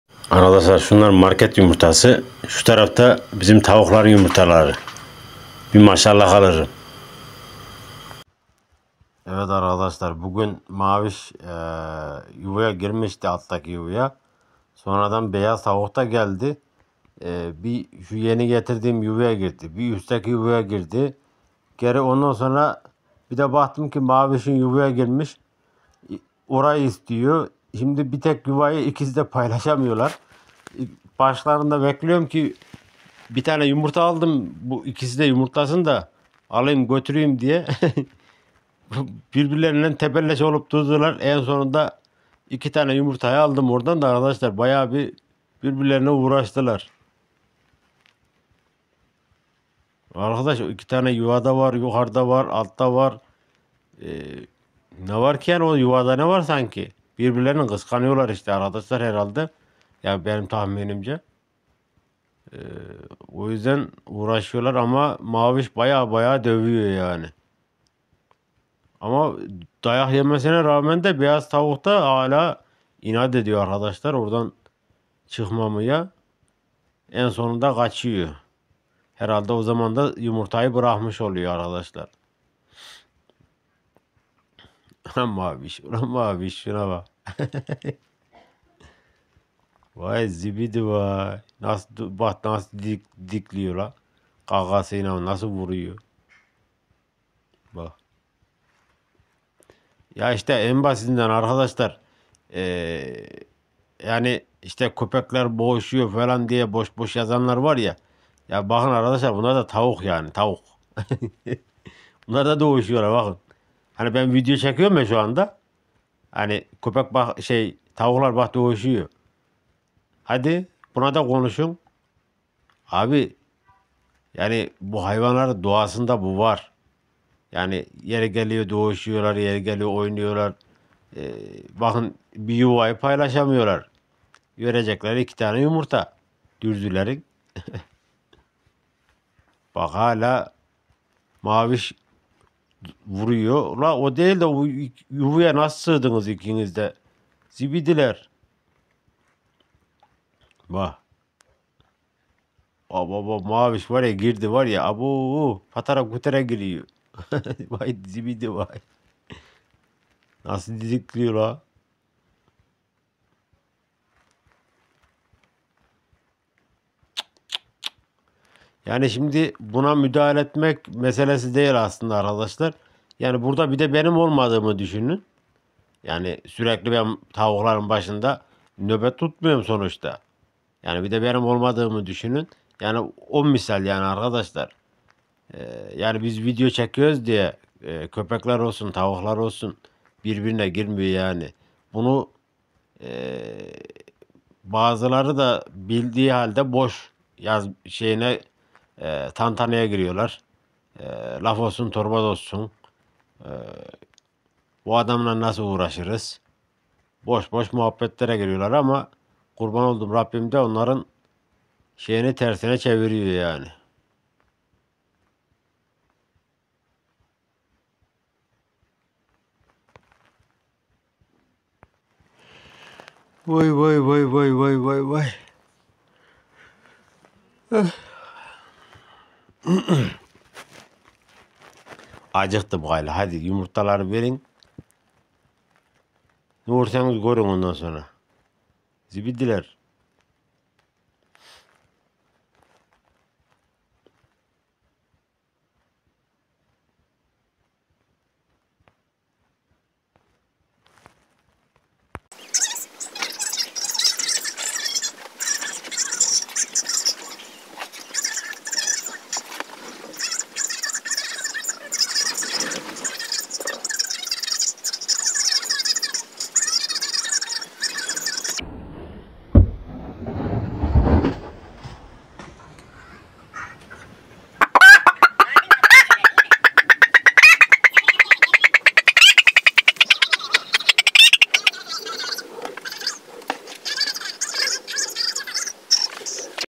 BİR YUVA İKİ TAVUK YUMURTLAMAYA GİRERSE KAVGA KAÇINILMAZ. 28 Ağustos 2024
Köpek ve tavuklar ile köyde yaşayan